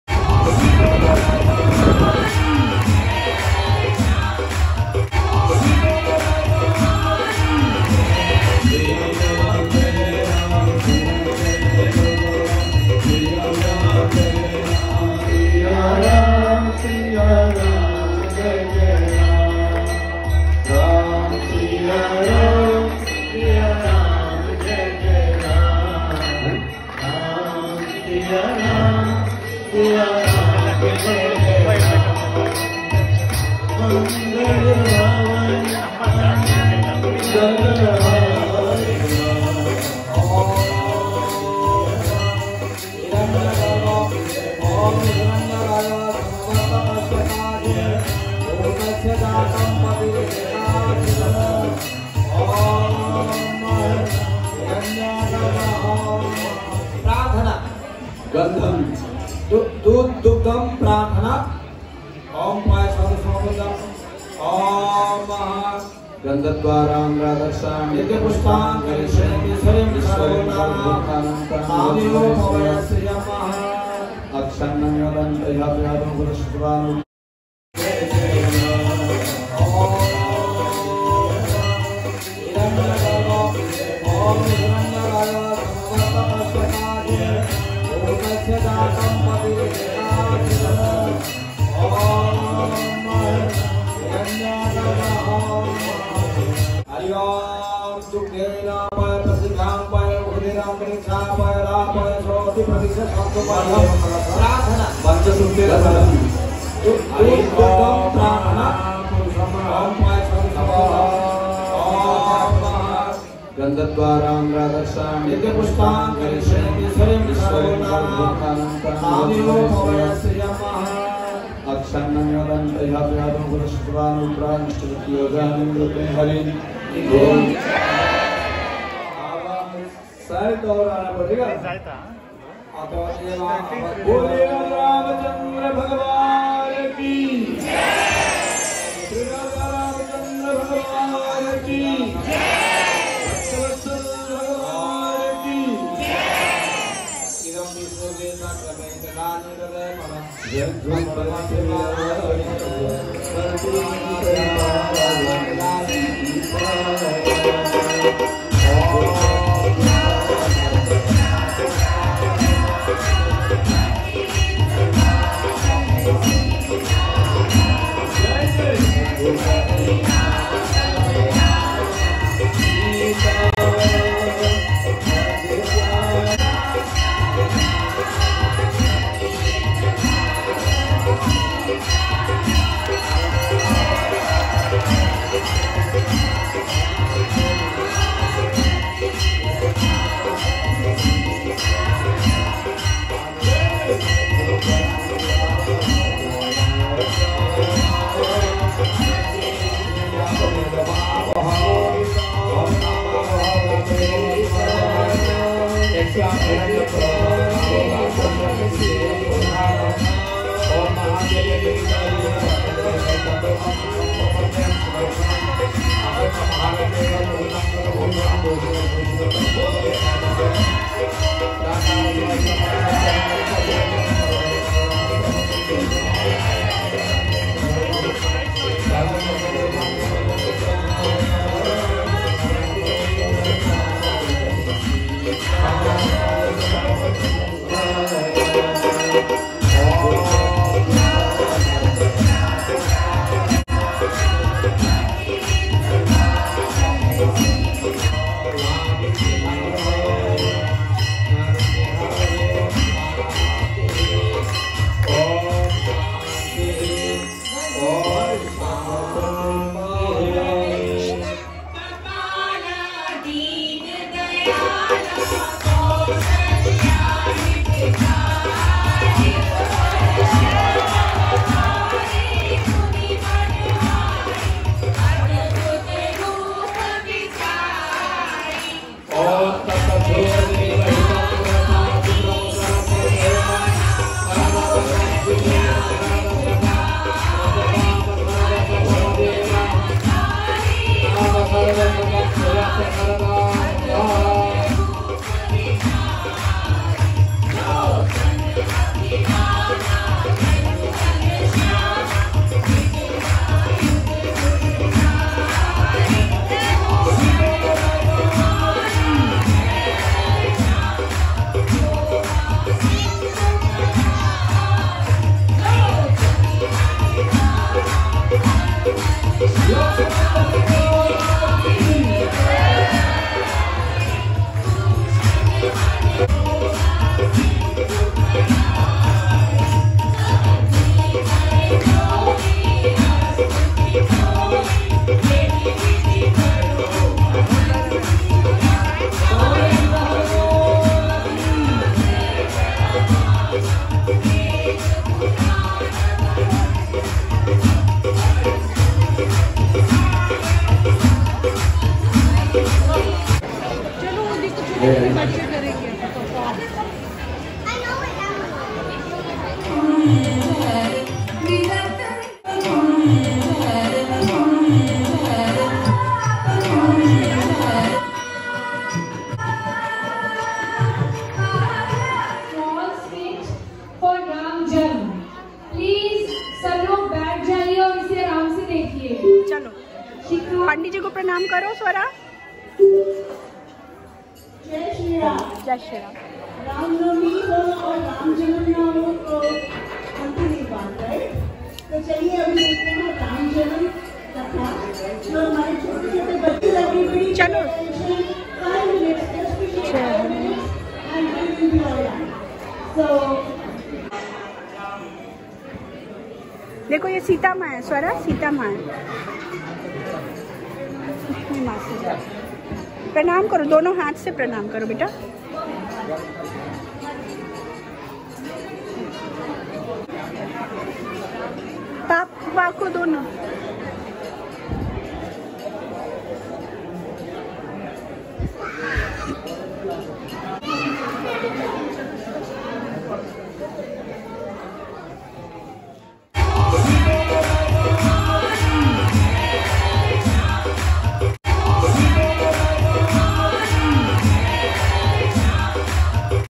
Ram Navami 2022 celebration in USA - Shri Ram Darbar Pran Pratishtha at Shri Ram Mandir @ Plano
Pran Pratishtha of Shri Ram darbar - Shri Ram, Mara Sita, Shri Lakshman and Bhakt Siromani Hanuman. Ram Navami Celebration